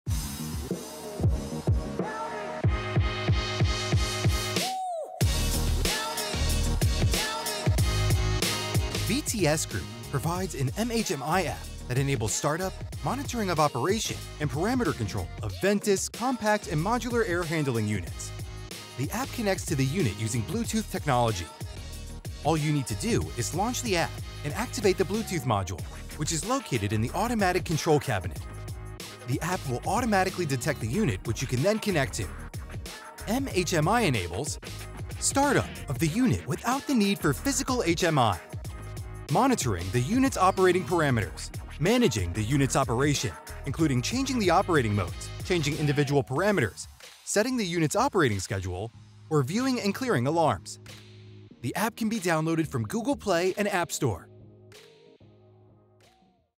Aplicatia mHMI
Grupul VTS oferă o aplicație mHMI care permite pornirea, monitorizarea funcționării și controlul parametrilor unităților compacte și modulare de tratare a aerului VENTUS.

Aplicația se conectează la unitate utilizând tehnologia Bluetooth. Tot ce trebuie să faceți este să lansați aplicația și să activați modulul Bluetooth, care se afl ă în dulapul de control automat. Aplicația va detecta automat unitatea la care puteți apoi conecta.

mHMI permite:
» pornirea unității fără a fi nevoie de un HMI fi zic
» monitorizarea parametrilor de funcționare a unității
» gestionarea funcționării unității, inclusiv schimbarea modurilor de funcționare, schimbarea parametrilor individuali, setarea programului de funcționare al unității sau vizualizarea și ștergerea alarmelor.

Aplicația poate fi descărcată din Google Play și App Store.